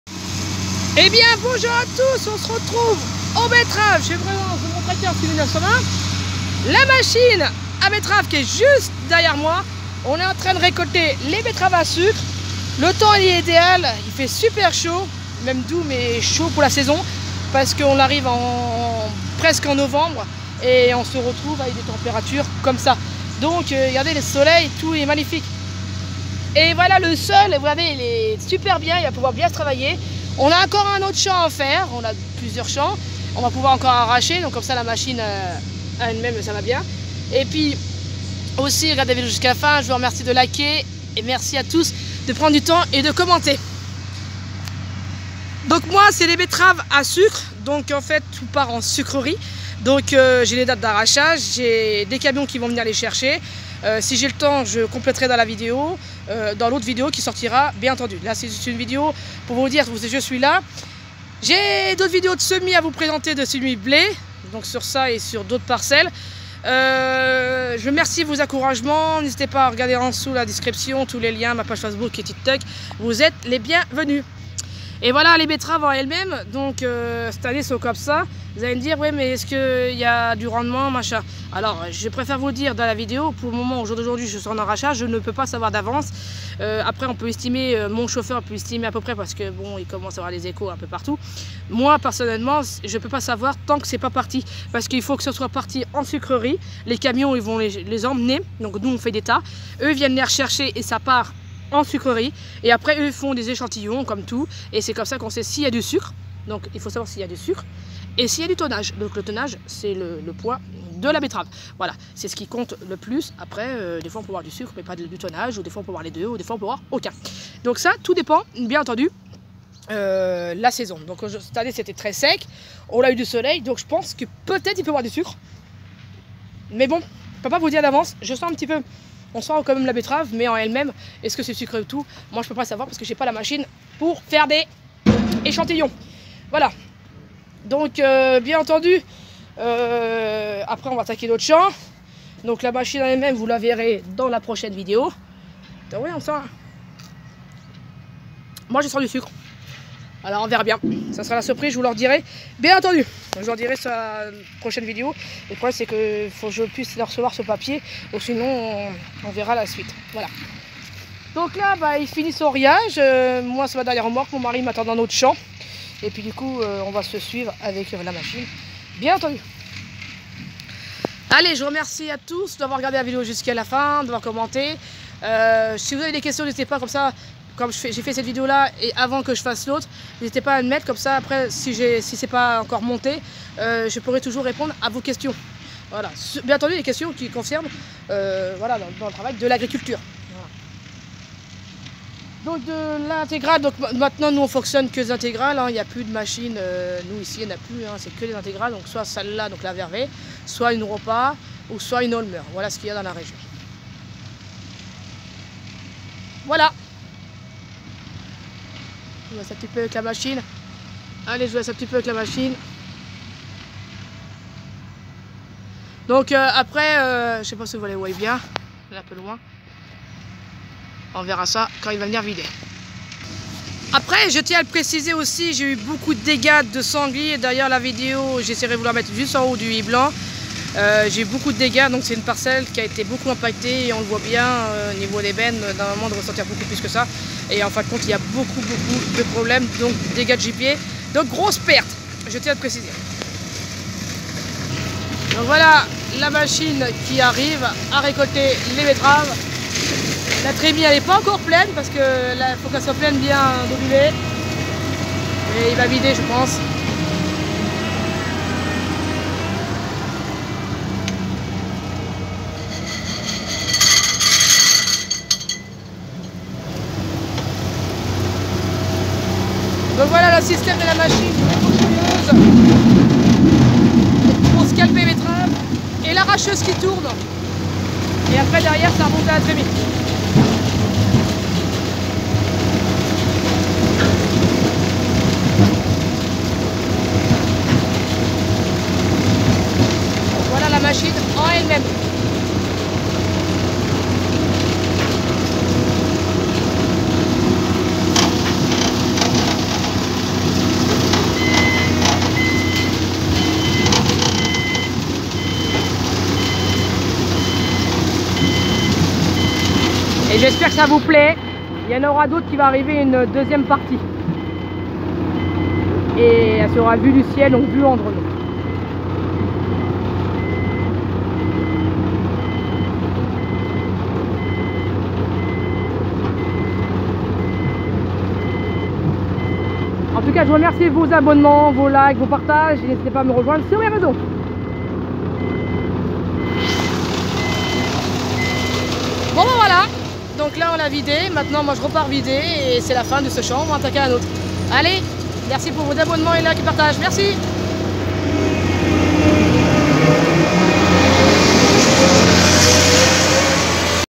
récolte betterave à sucre année 2022 avec machine à betterave vervaet
merci a vous a bientôt